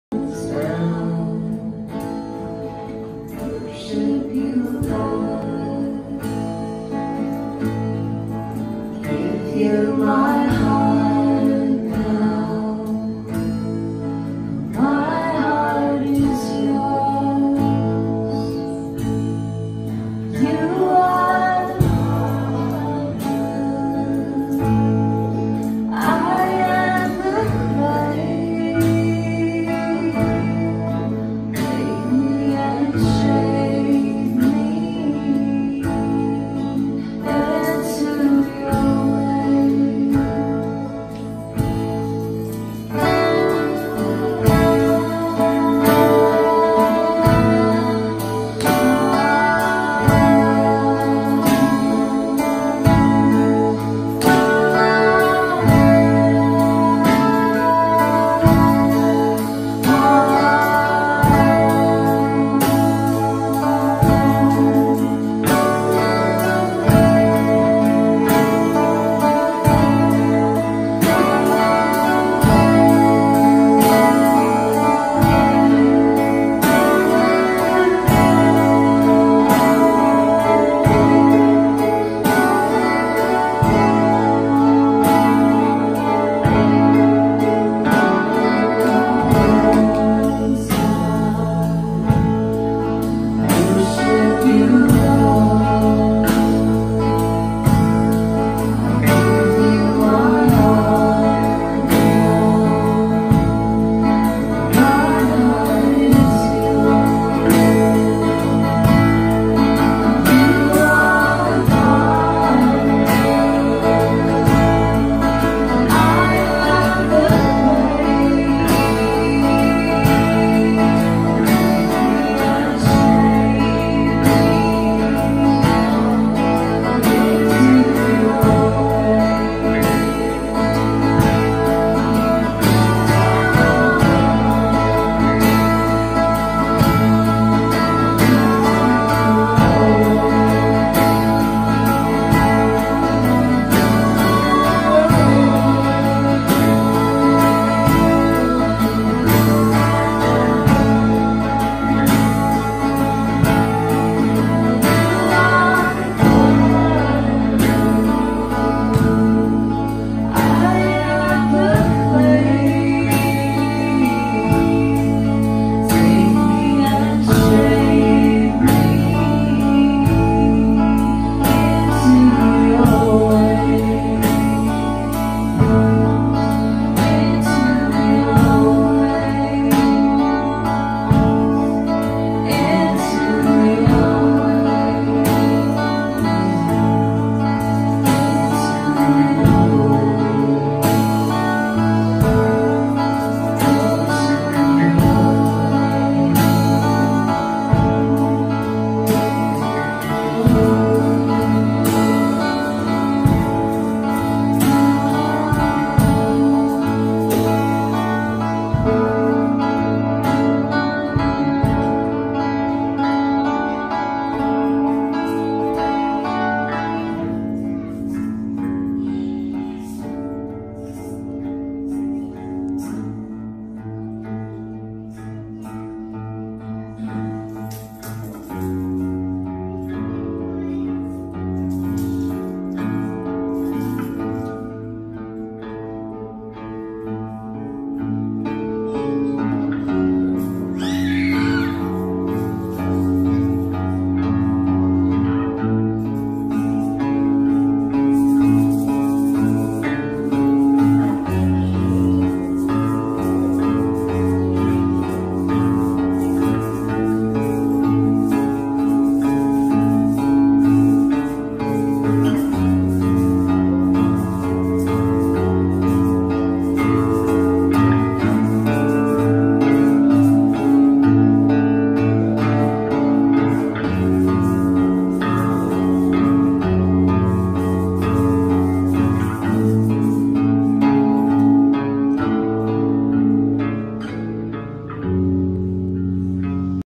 You Are the Potter - Morris Family
Freedom Church - August 4, 2019
Sermon - August 4, 2019: By David Ryan Engerer - Faith

Bible Study - July 30, 2019: By Jeremy Morris - Matthew 28